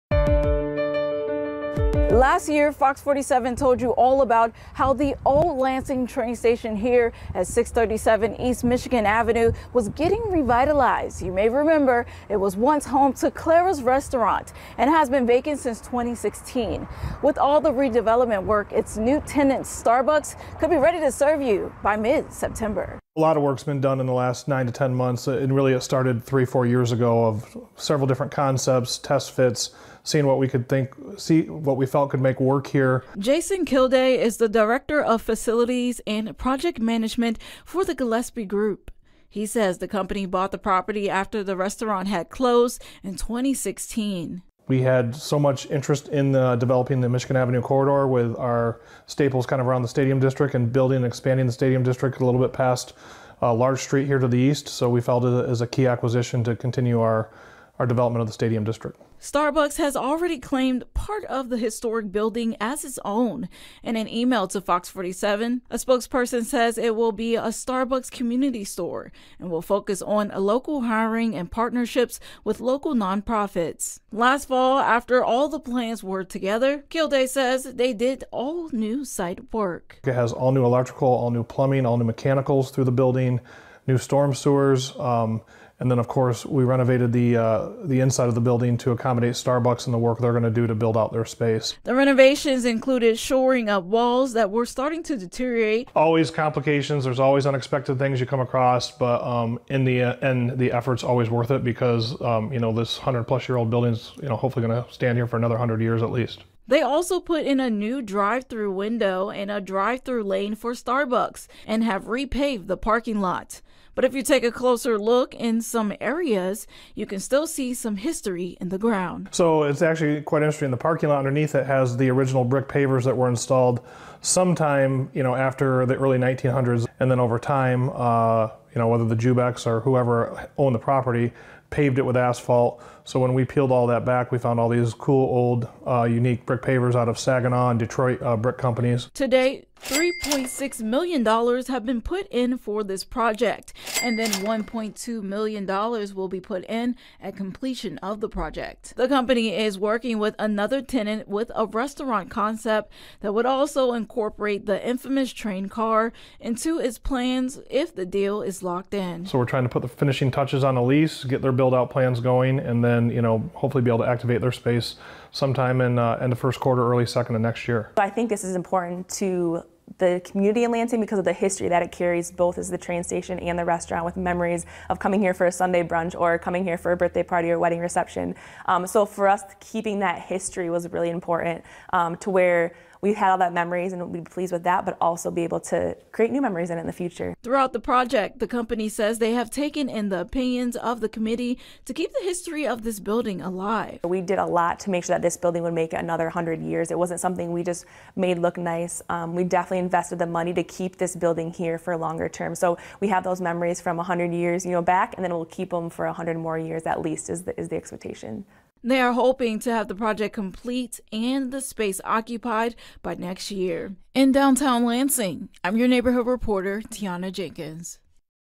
Renovations underway for old train station in Lansing
A lot of work has been done to revitalize the old Lansing train that sits at 637 E. Michigan Avenue since last fall. Starbucks has already claimed part of the historic building as its own.